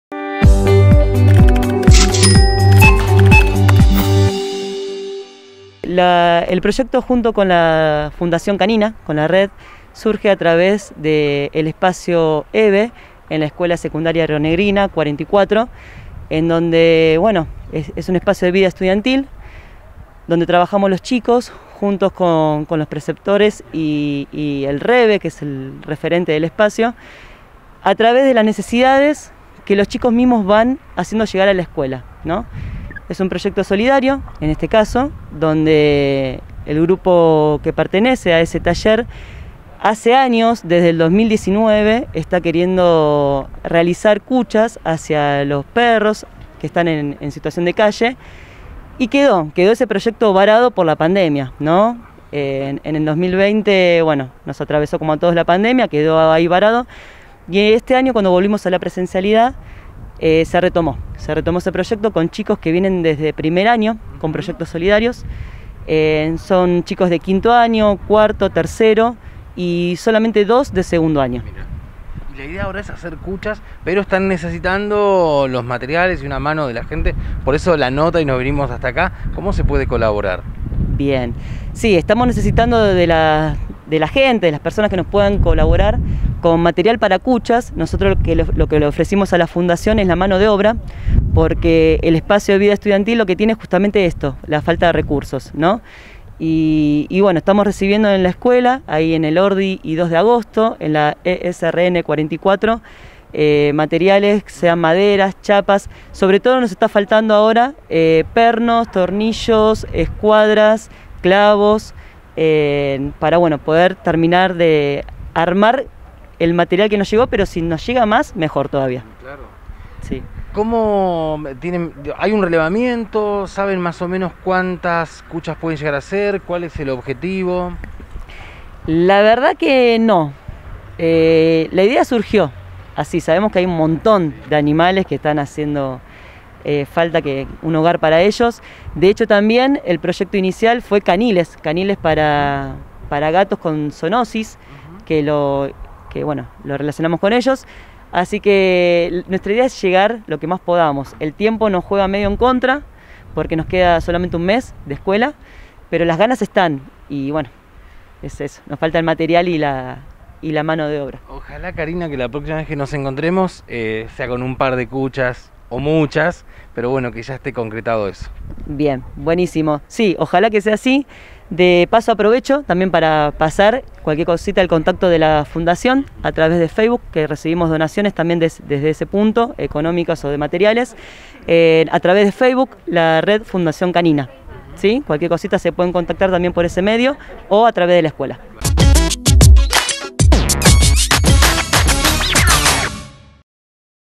Un proyecto de estudiantes apunta a la fabricación de cuchas para perros en situación de calle
El trabajo se puso en marcha. A cargo del mismo se encuentran alumnos de la Escuela Secundaria Rionegrina en forma conjunta con la Fundación Red Canina Bariloche. Piden a la comunidad la donación de materiales. Karina Rogers dialogó con Noticiero Seis contando detalles de la iniciativa.